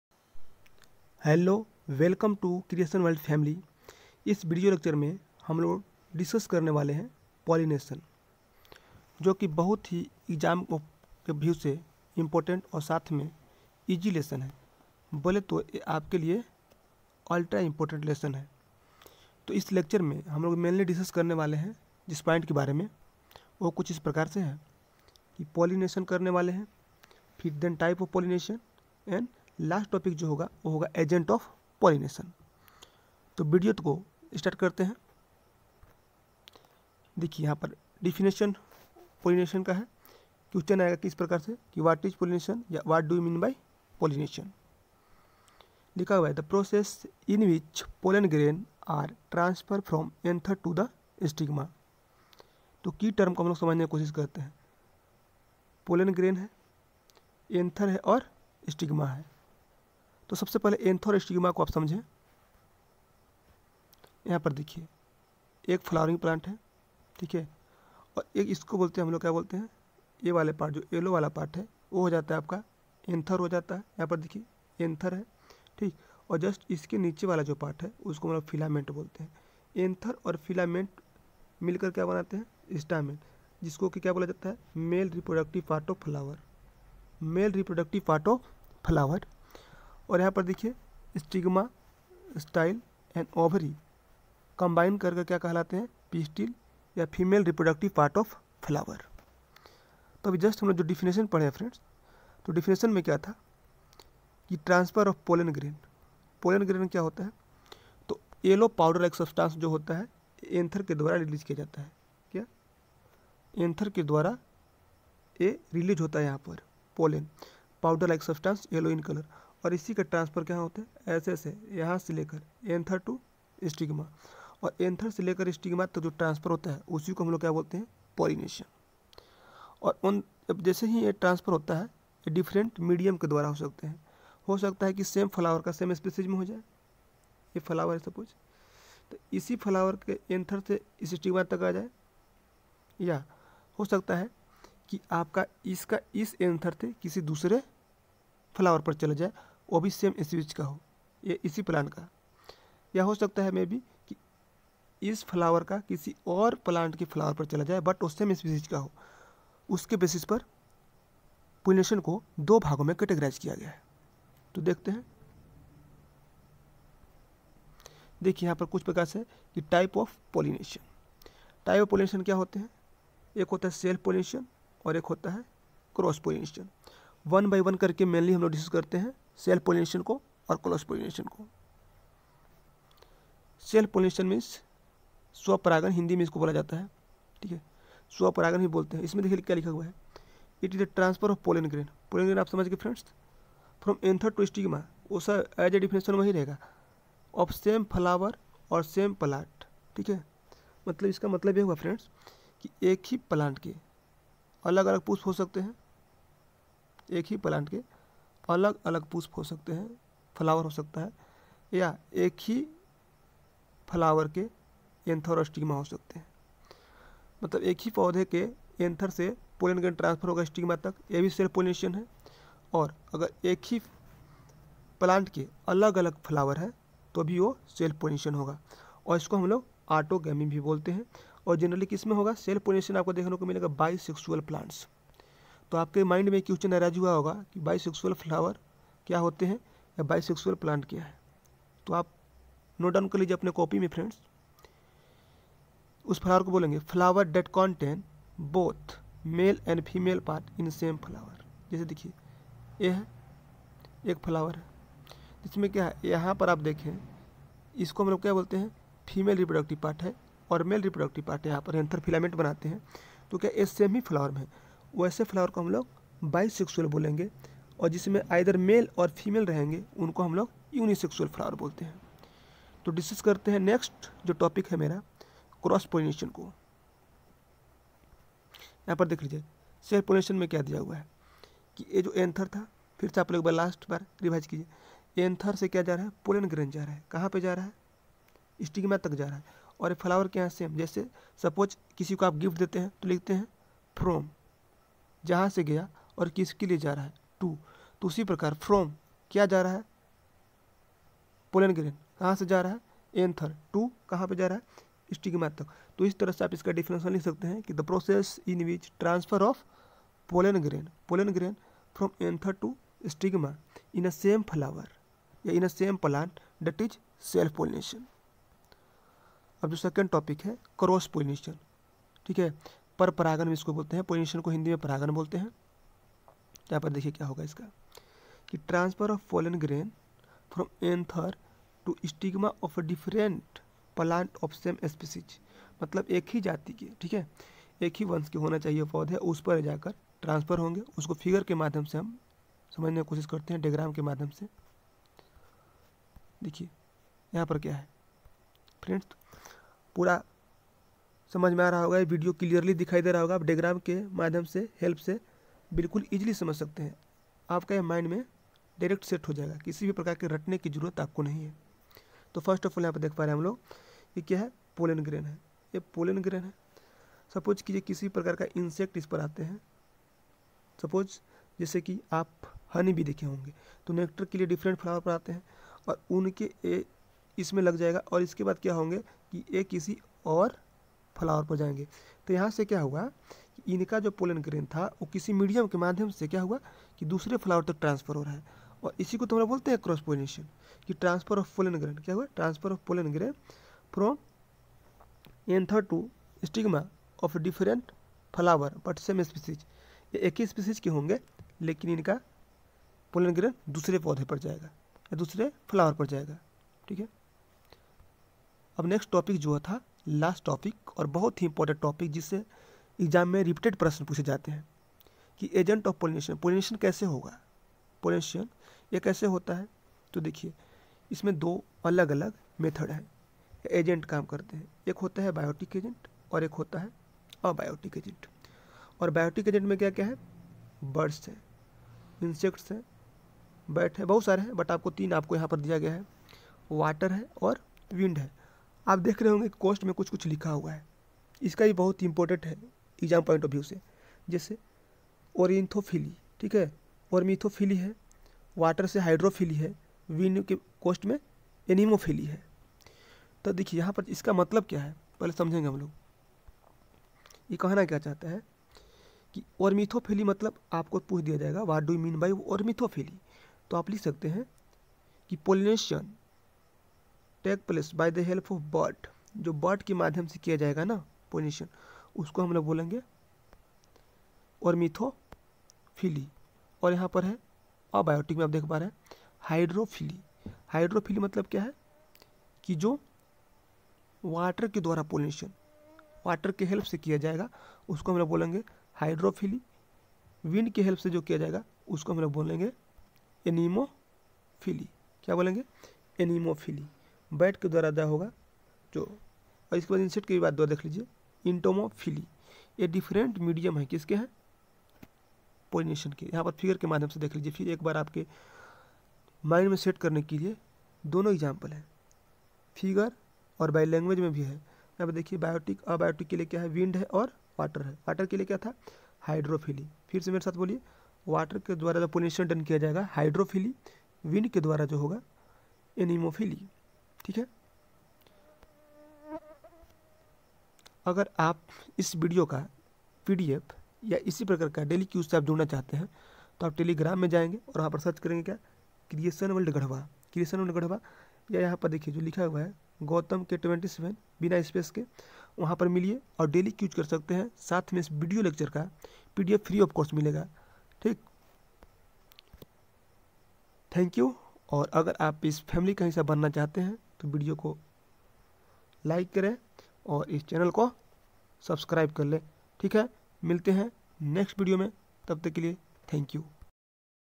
Pollination ।। Reproduction in flowering plants।। Science ।। Biology। परागण
Hello.
Welcome to Creation World Garhwa family
In this video lectures mainly  discuss about Pollination and type of pollination.

❤️Thanks❤️